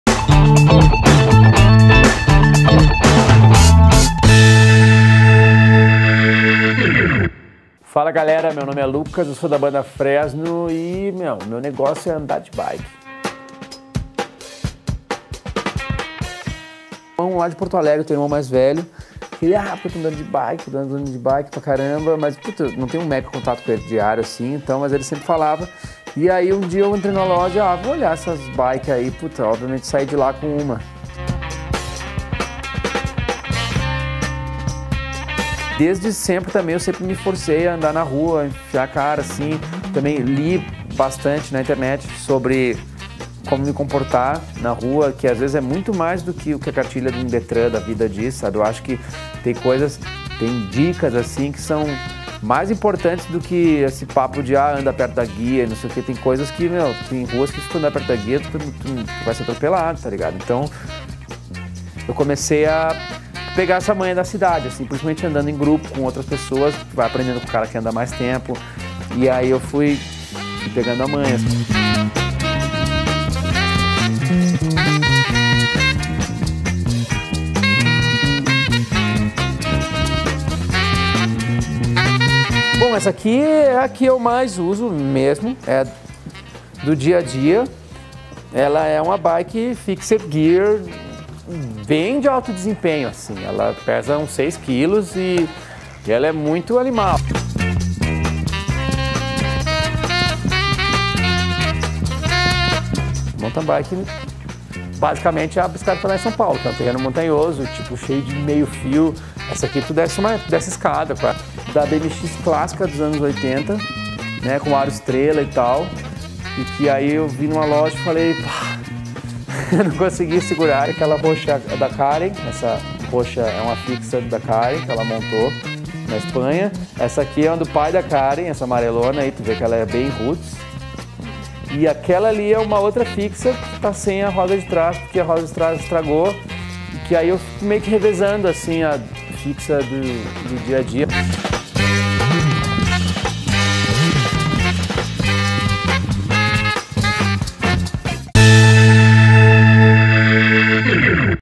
Lucas Silveira, da Fresno, fala da sua paixão por bikes
Vocalista recebe a equipe do Oxigênio TV para mostrar sua coleção de bicicletas. Confira!